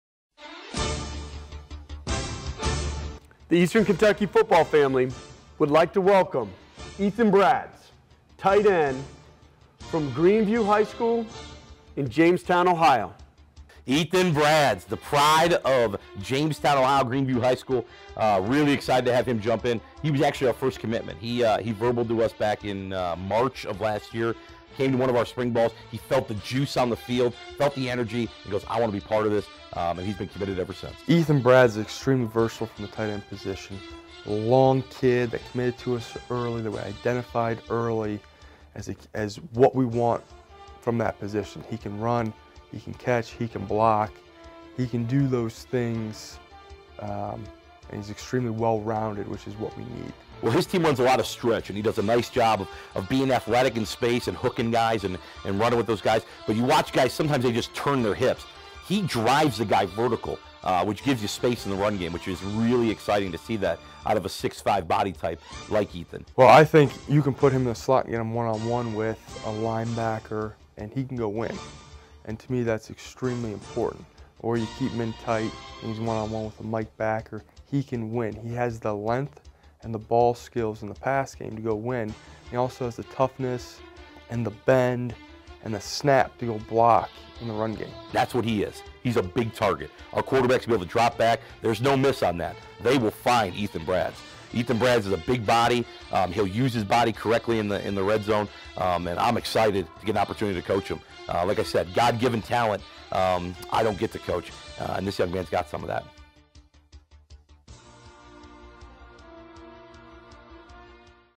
EKU Football National Signing Day - Ethan Bradds
Head coach Mark Elder introduces Colonel fans to tight end Ethan Bradds.